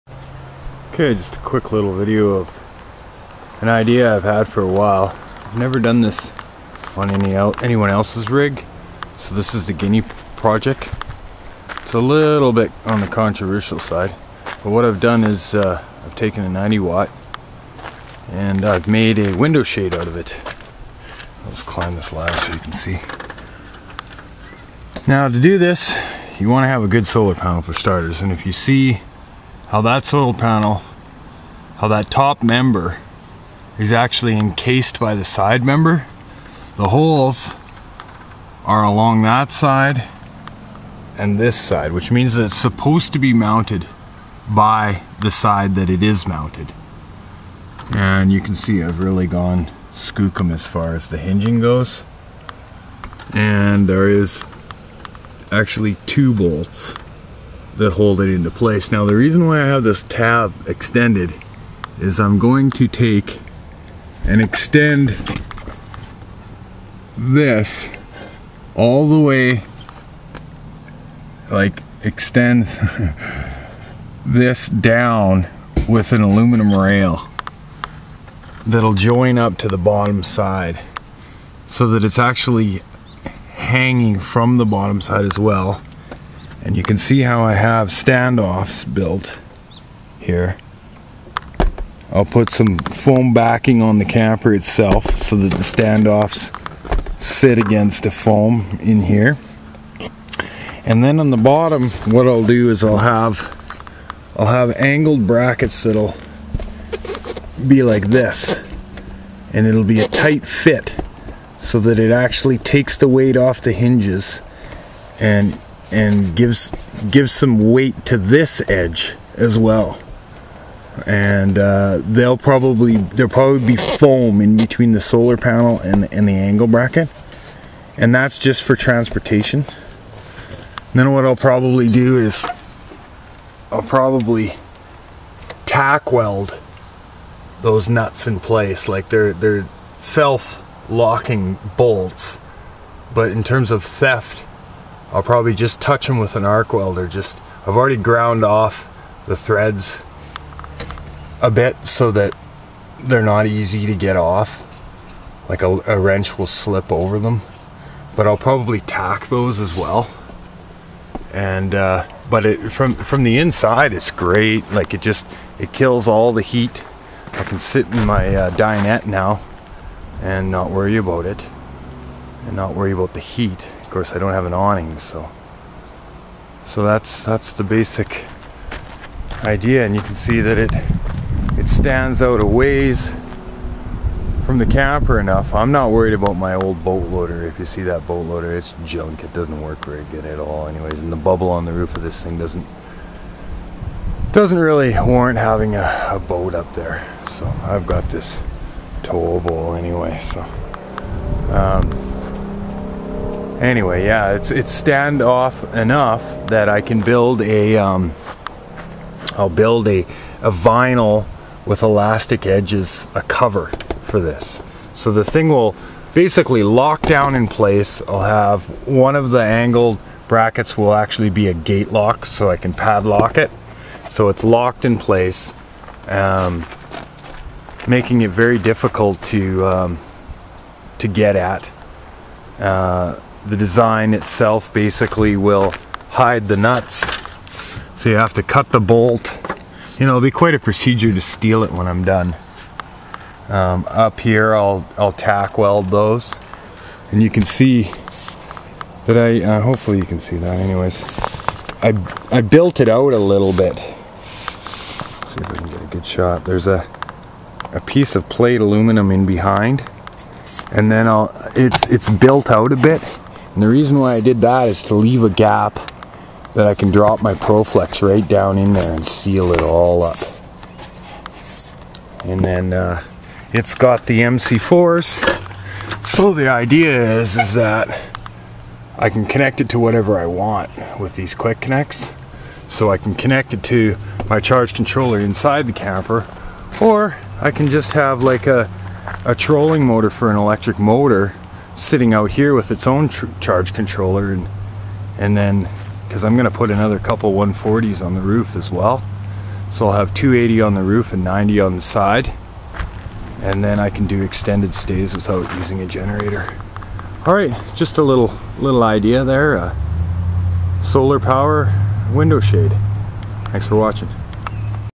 RV Solar Panel Mounting - Building a Window Shade out of 90 Watt Monocrystalline Panel
This is an idea I have had for a while, I've never actually seen it done, but I'm sure someone has done it! I thought I'd do my own, first and see how it works out before installing panels in this manner, on other people's RV's.